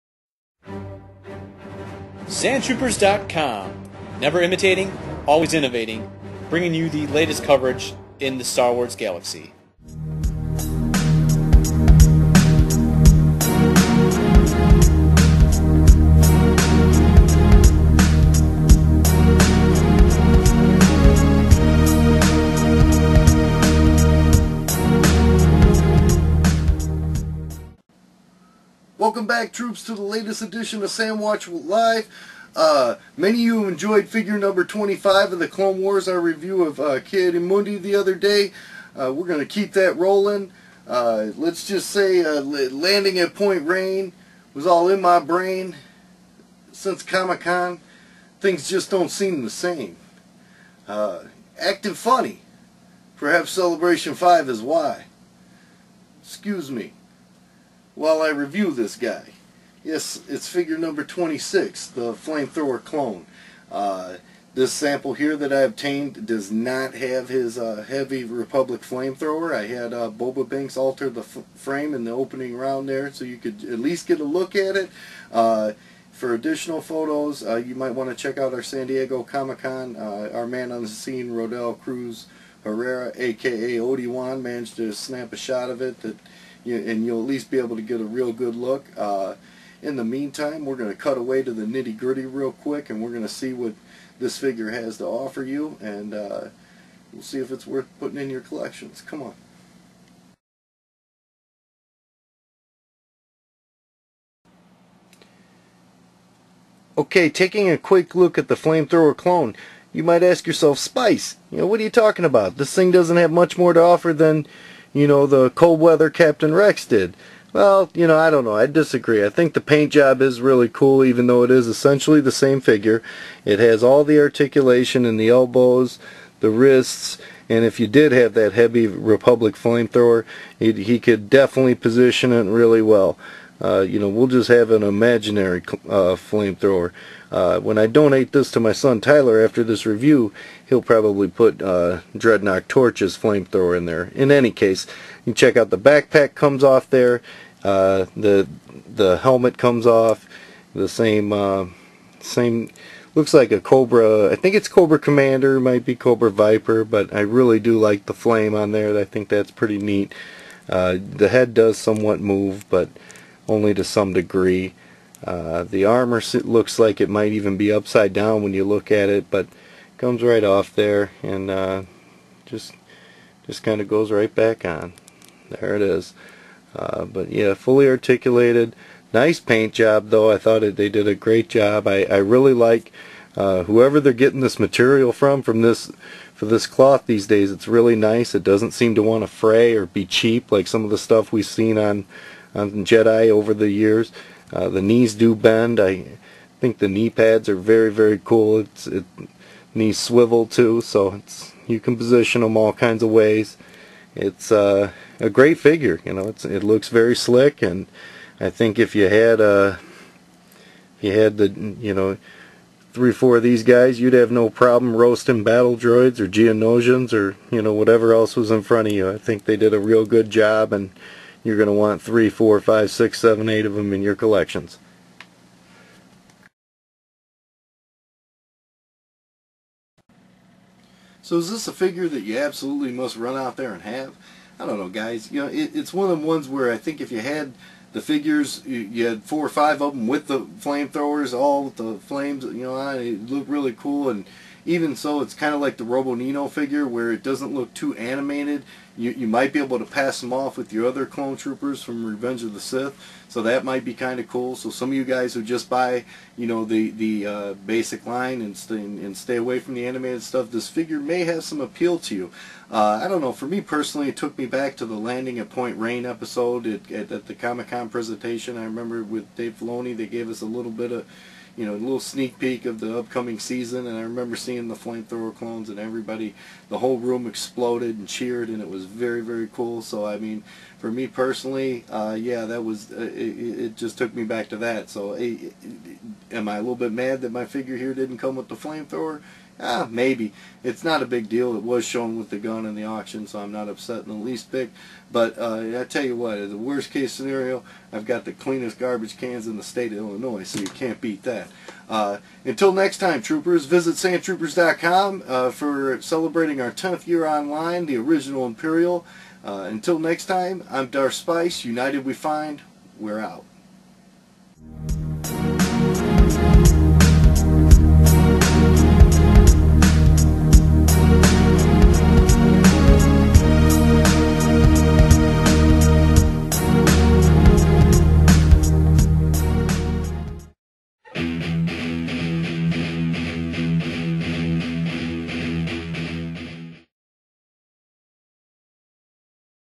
Flame Thrower Clone Trooper #26 - CW 2010
We follow up our review of Ki-Adi Mundi with the very Troopers that bailed him out at Point Rain. Long before the Galactic Marines did him in, the Flame Thrower Clone Troopers escorted the Jedi General through a "hot" landing zone.  How does the action figure stand up? Our reviews continue to sizzle as we take a look at another great figure from the upcoming wave three.